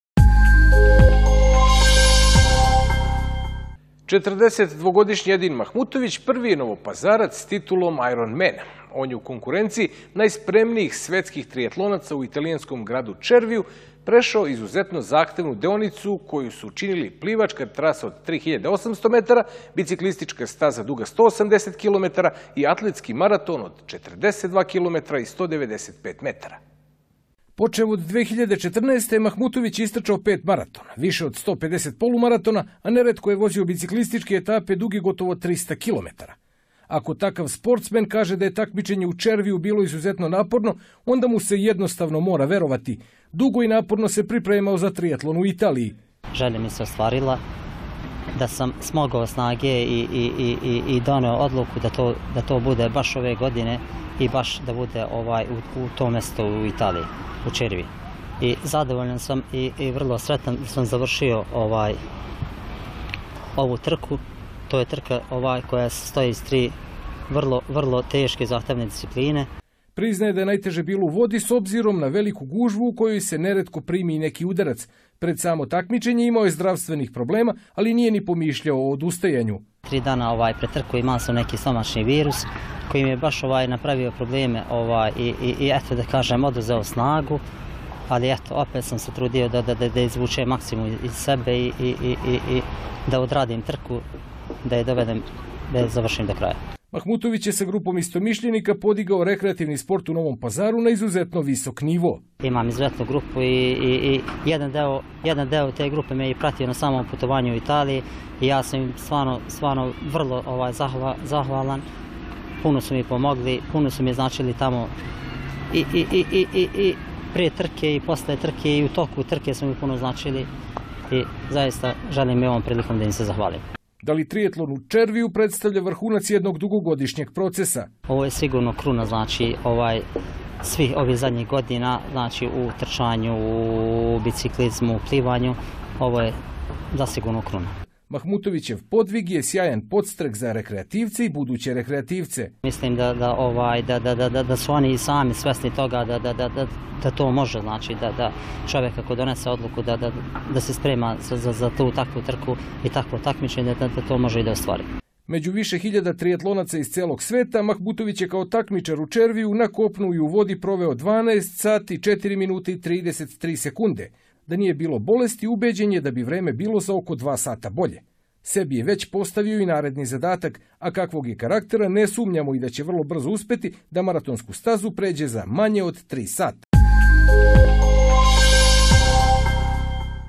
Ironman Edin Mahmutović
Četrdesetdvogodišnji Edin Mahmutović prvi je Novopazarac s titulom Ironmana. On je u konkurenciji najspremnijih svetskih trijatlonaca u italijanskom gradu Červiju prešao izuzetno zahtevnu deonicu, koju su činili plivačka trasa od 3.800 metara, biciklistička staza duga 180 kilometara i atletski maraton od 42 kilometra i 195 metara.